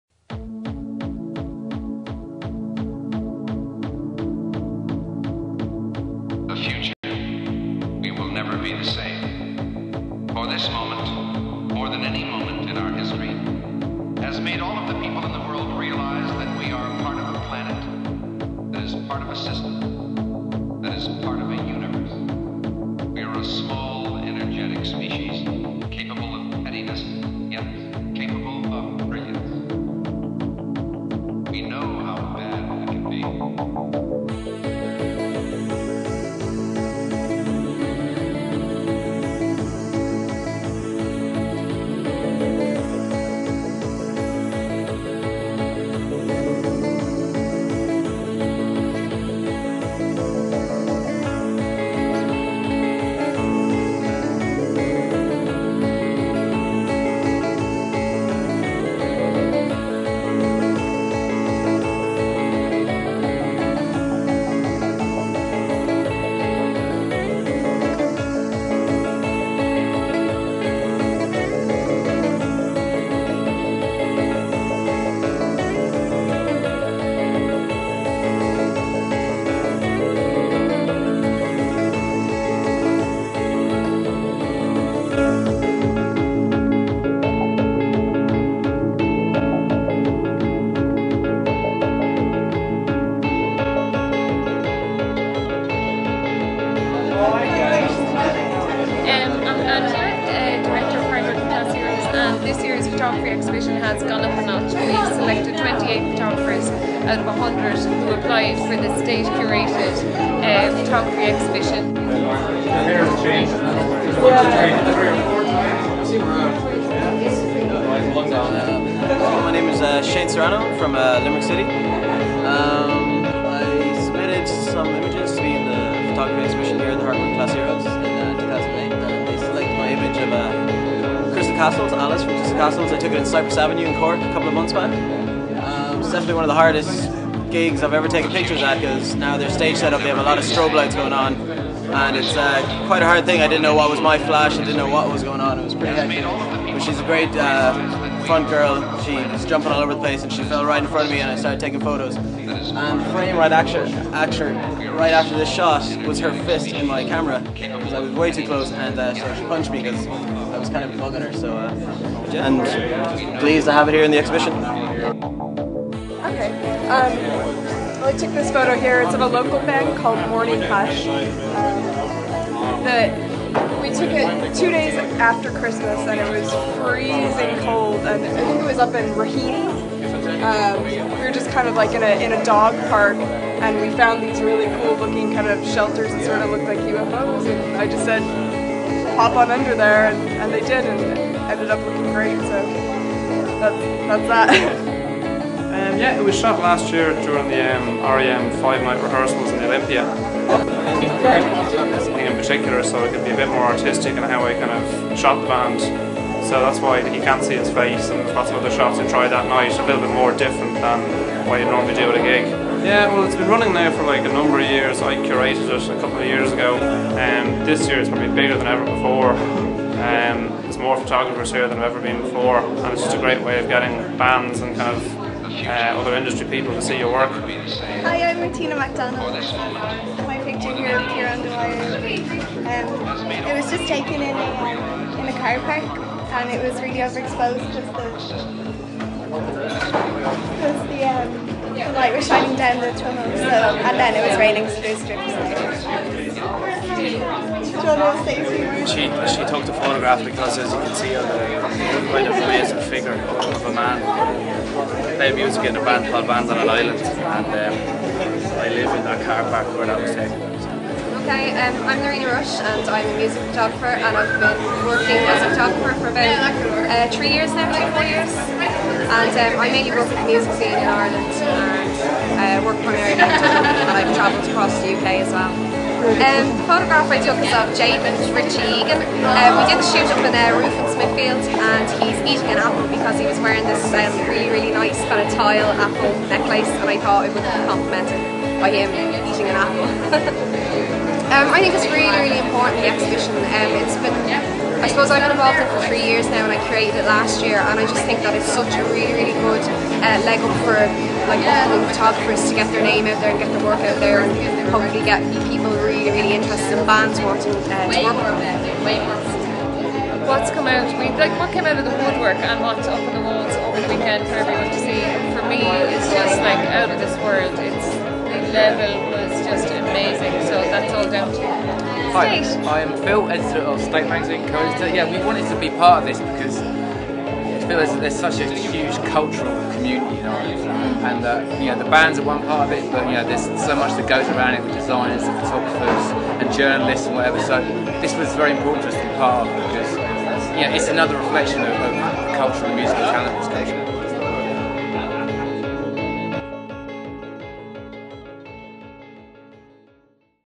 Photography Exhibition @ HWCH 2008
My Edit of the Photography Exhibition @ HWCH 2008, with Le Galaxie soundtrack